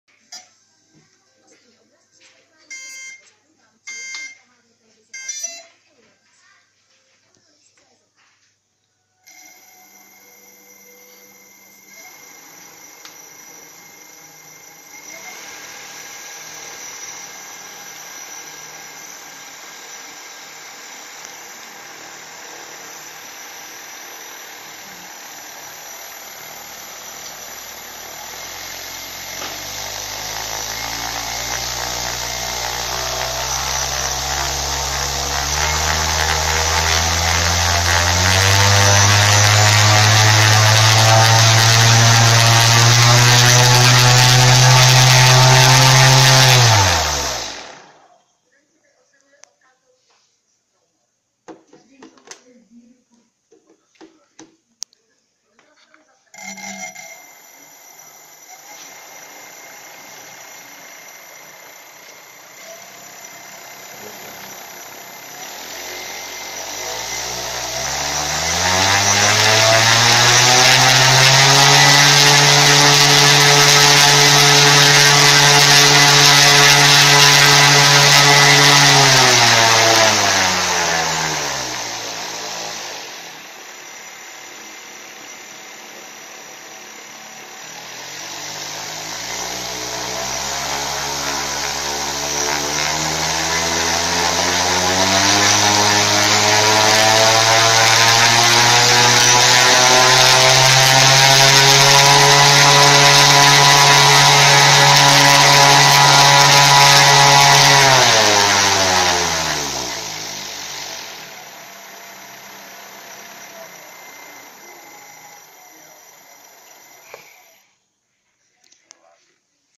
Homemade rc brushlees 320kv 2
Druhá zkouška , vykon s vrtulí 21/10  1500w, dalši zkouška s vrtulí 25/8